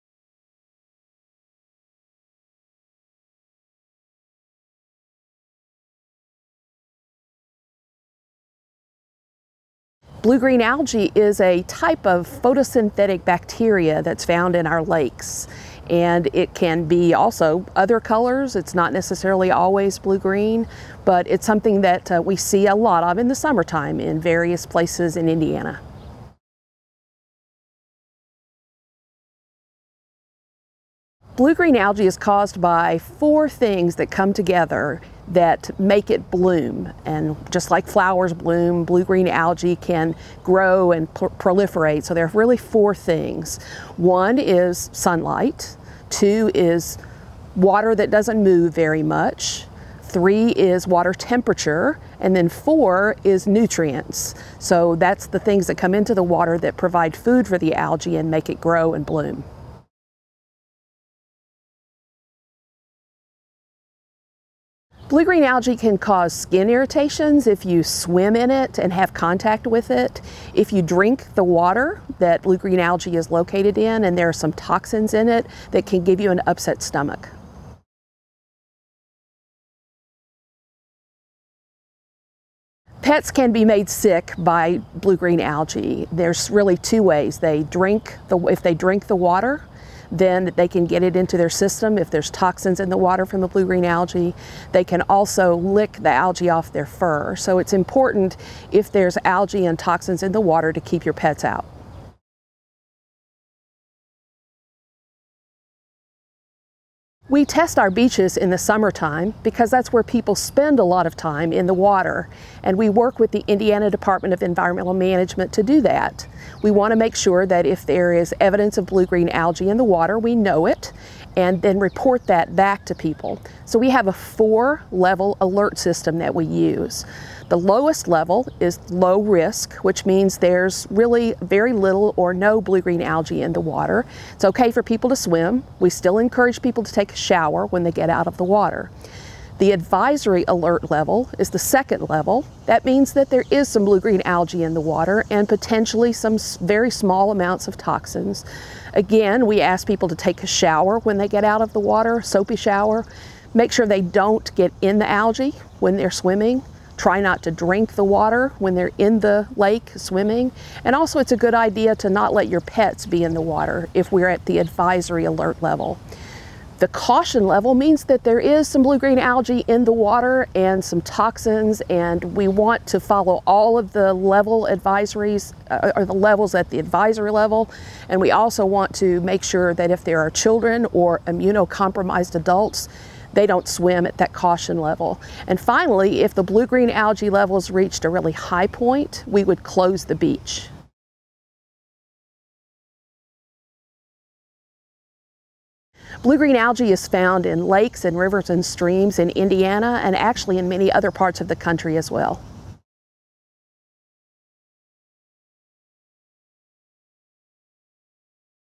Blue-Green Algae Q&A
Indiana State Parks Deputy Director Ginger Murphy answers a few questions about Blue-Green Algae in Indiana.